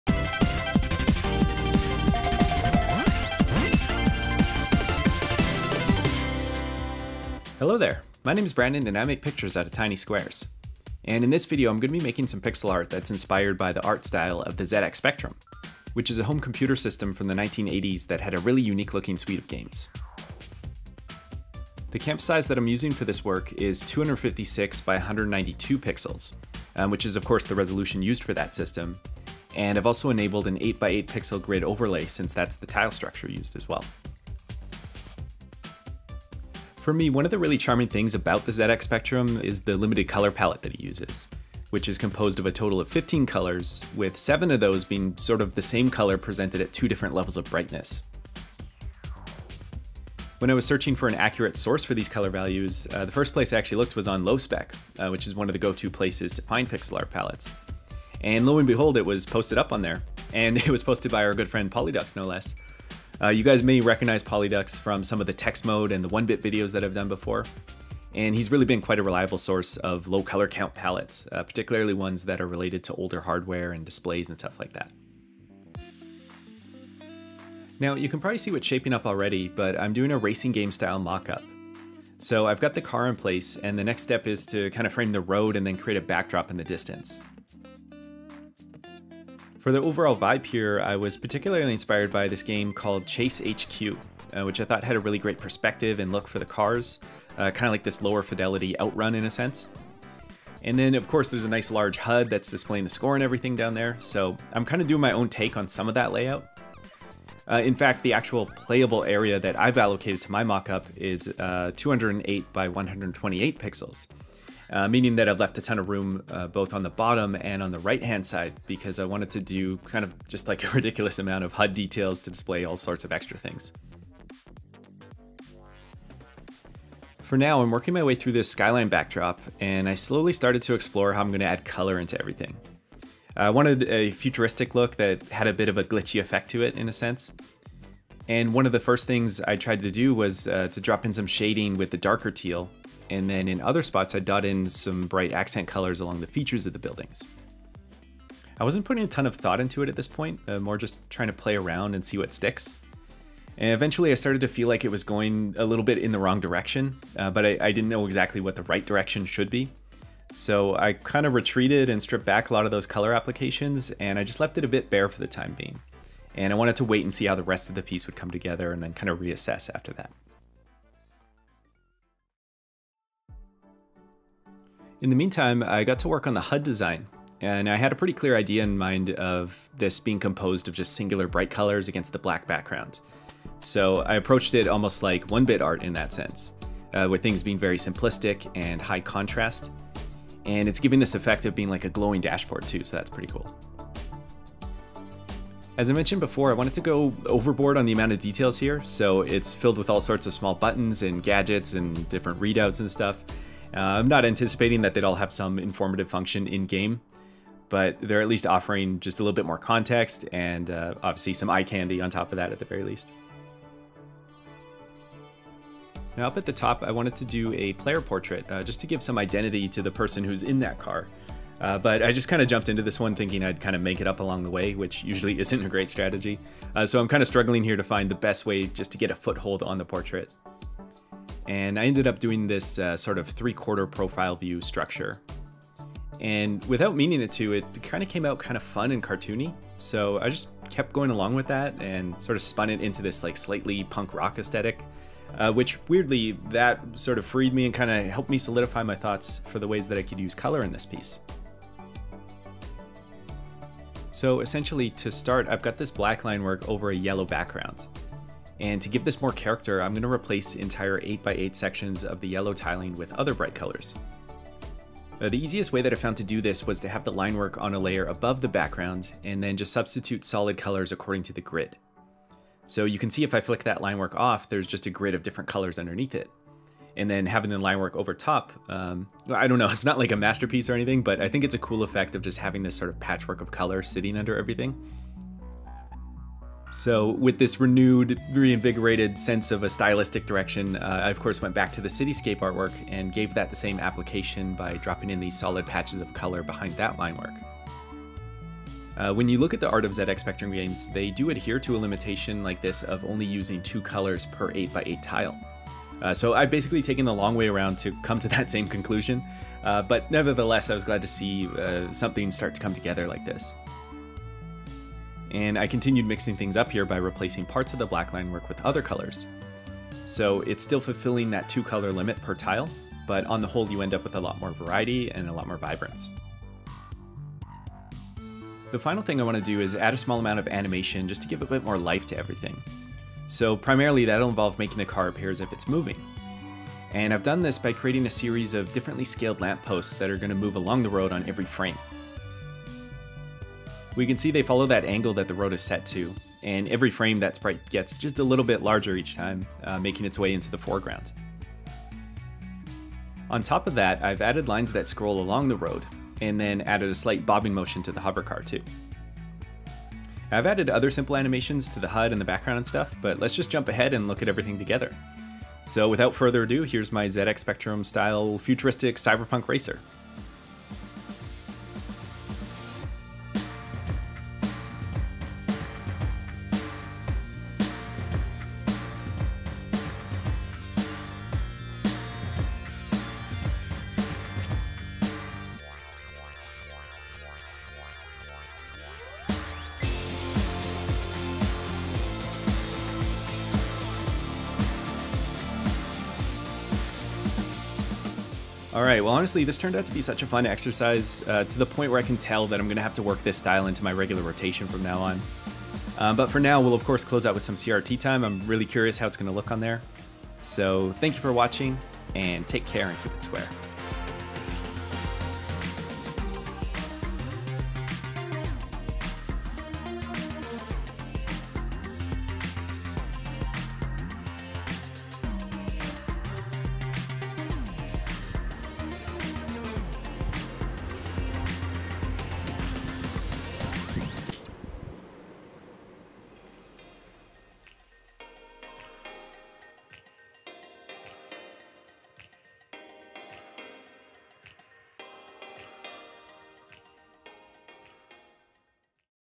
Making Pixel Art Inspired by the ZX Spectrum!
In this video, I’m exploring the unique look of ZX Spectrum games - and this style quickly became one of my favourites! Follow along as I detail the colour palette, inspirations, and up-and-downs of putting together my first ZX Spectrum artwork!

0:00 Intro
0:21 Canvas Size + Colour Palette
1:16 Style + Inspiration
2:53 HUD Design
4:46 Finishing Details + Animation
6:10 FINAL ARTWORK
6:46 CRT Time
7:16 VID OVER

Software used: Adobe Photoshop 2022